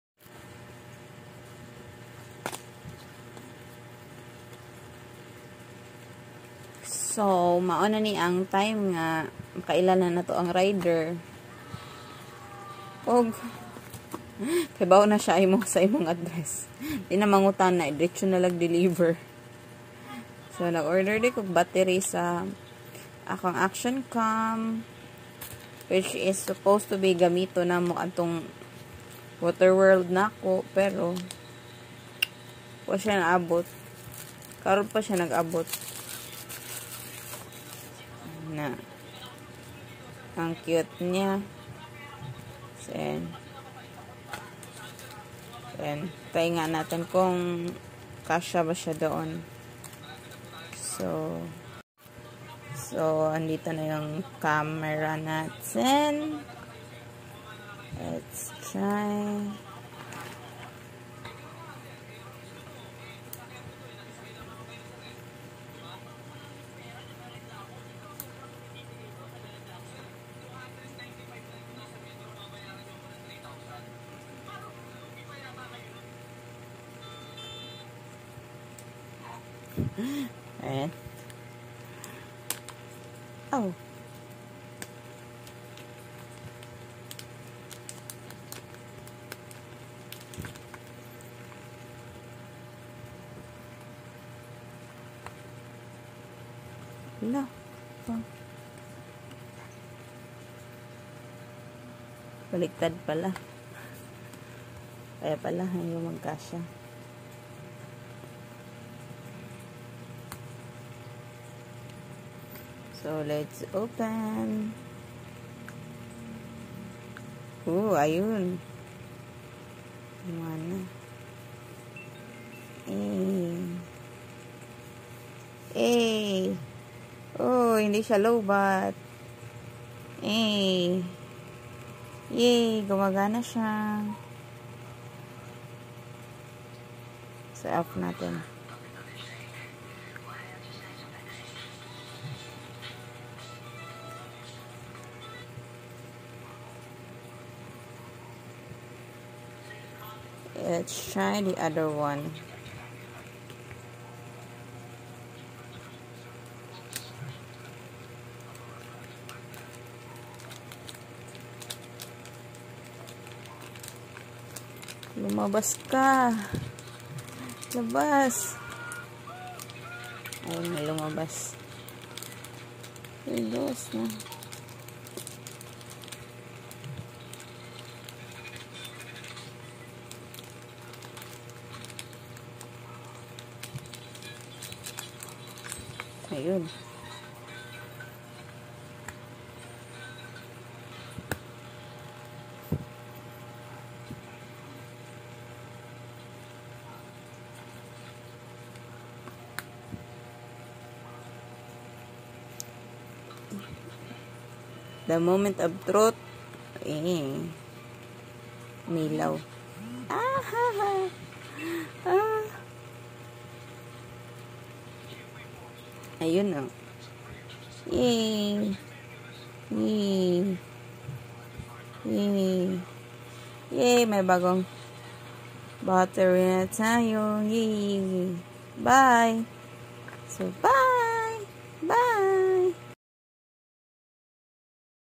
SJCam Battery and Charger Unboxing and Testing. Tina Lepaopao
Battery and charger unboxing for my SJCam Action camera.